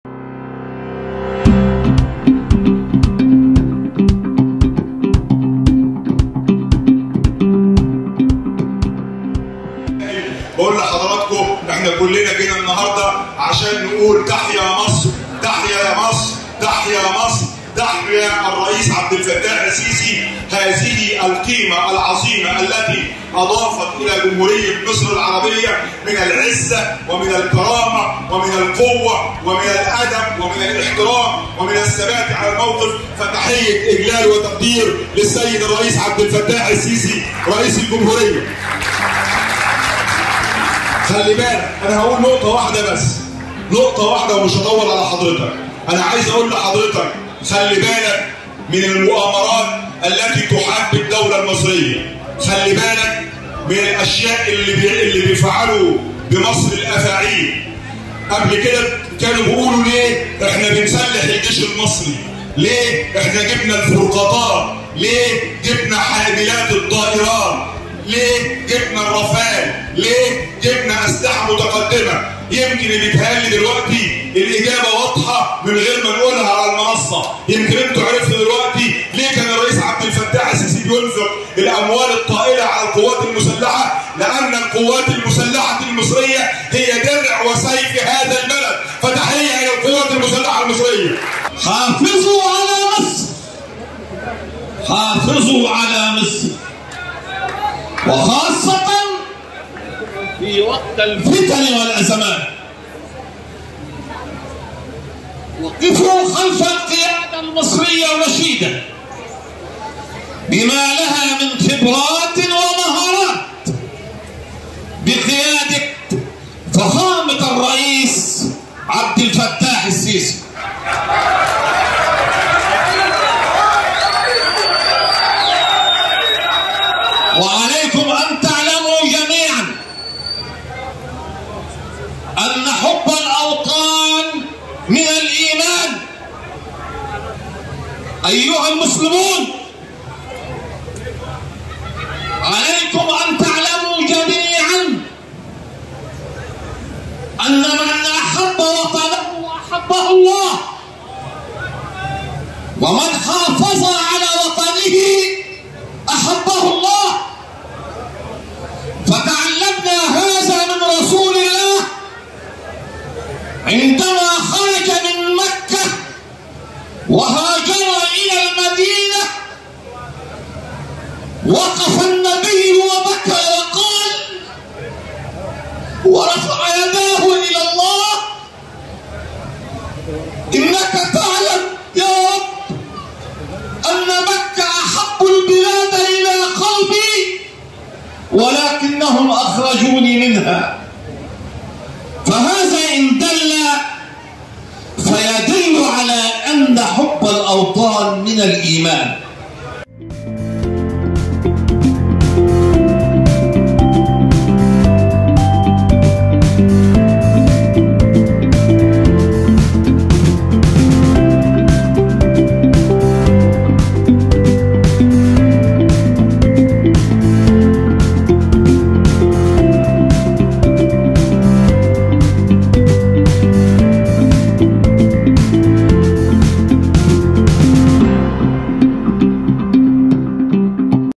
حضور جماهيري حاشد في مؤتمر دعم المرشح الرئاسي عبد الفتاح السيسي بالشرقية
تقرير : نظيمة البحراوي